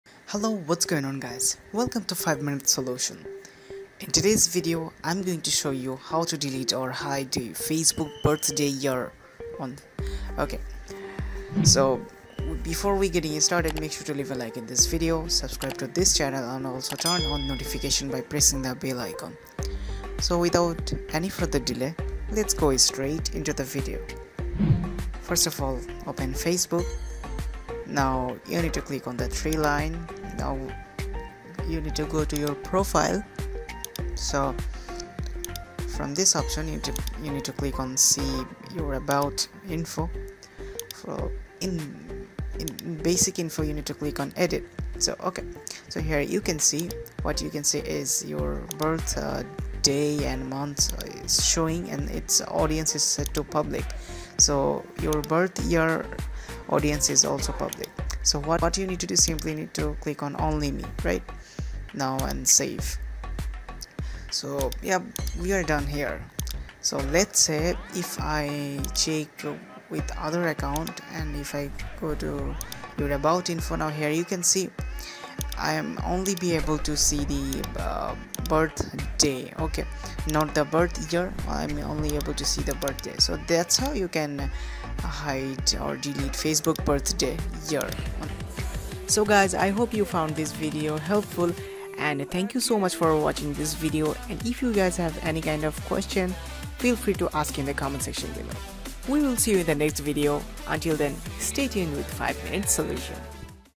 How to delete birthday on facebook 2024 | Hide Fb Birthday
How to Delete Your Date of Birth on Facebook
Log in to Facebook and click your name at the top of the screen to view your Timeline.
Click "About" beneath your profile photo.
Click the "Edit" button in the Basic Info section.
Click the drop-down field under your birthday and click "Don't Show My Birthday on My Timeline."